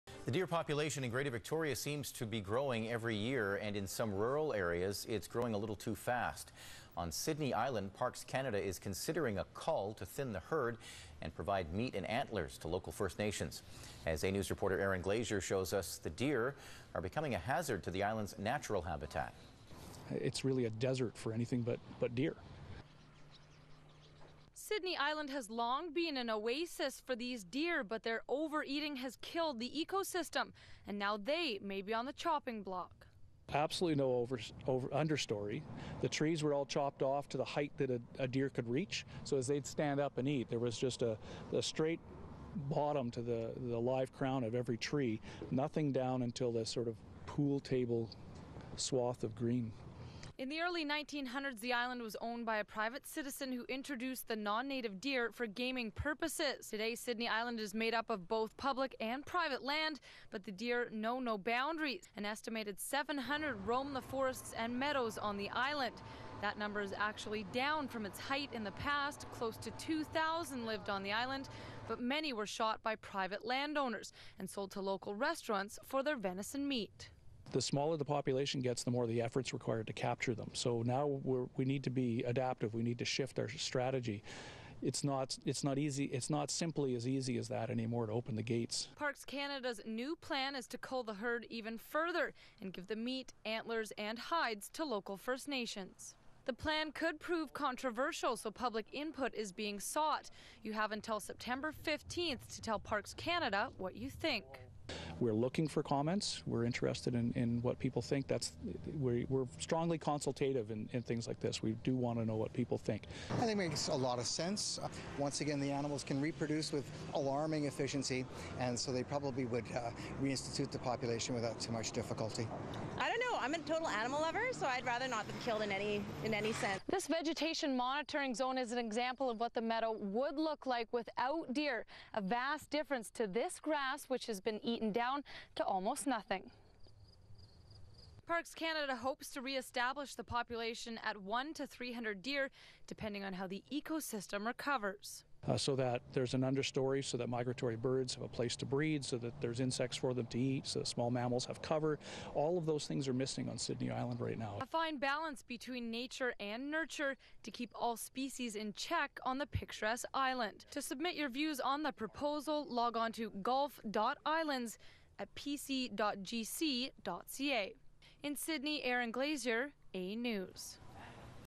Parks Canada Considering Deer Cull on Sidney Island
The deer population in Greater Victoria seems to grow every year and in some rural areas it's growing too fast.

On Sidney Island, Parks Canada is considering a cull to thin the herd and provide meat and antlers to local First Nations.